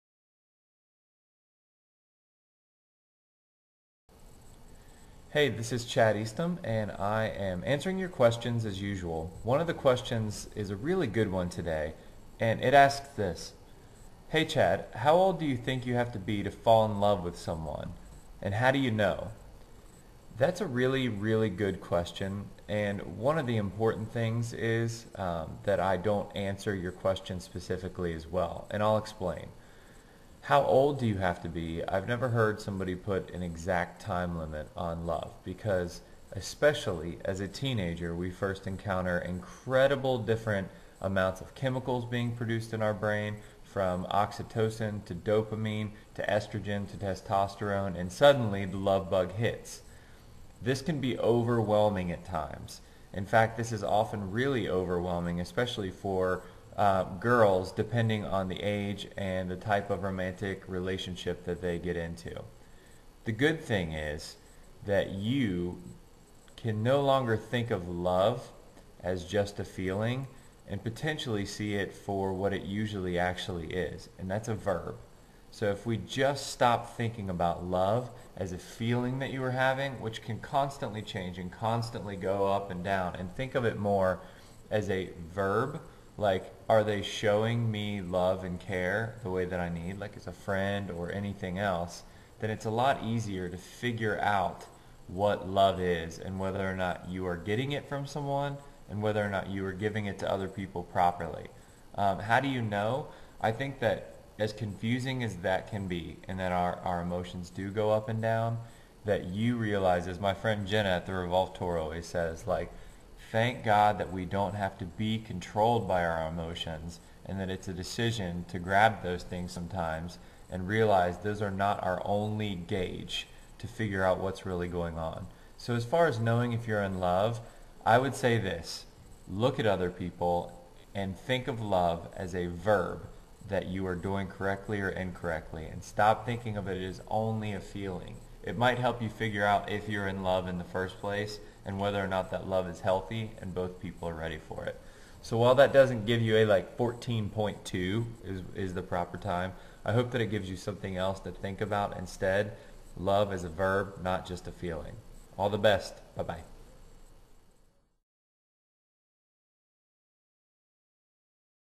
Ask Chad: How old do you have to be to fall in love with someone?
You asked, Chad answered.
How old do you have to be to fall in love with someone?